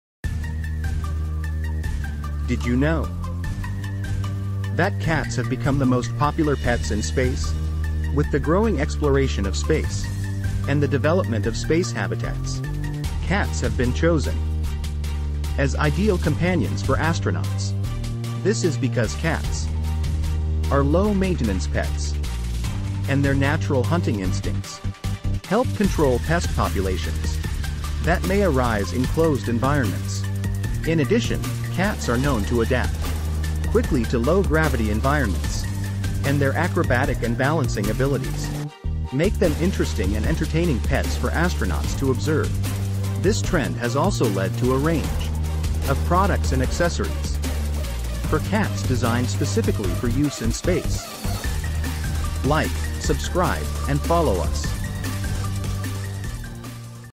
😺🚀 Cats in Space: The Ultimate Astronaut Companions! 🌌🐾
Get ready to blast off on this incredible journey through space with our feline friends! 😻 Discover why cats have become the most popular pets among astronauts and how they adapt to life in space. 🌠 Also, check out the latest accessories and products designed specifically for space cats!